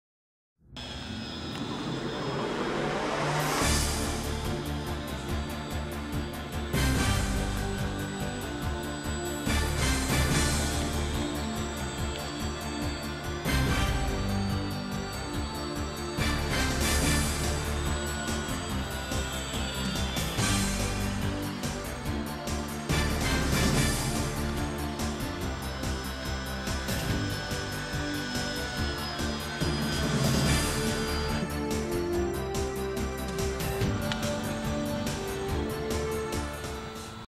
Stingray Elektro Erstflug
Erstflug mit meinen Stingray Elektro aus Depron mit Frontantrieb!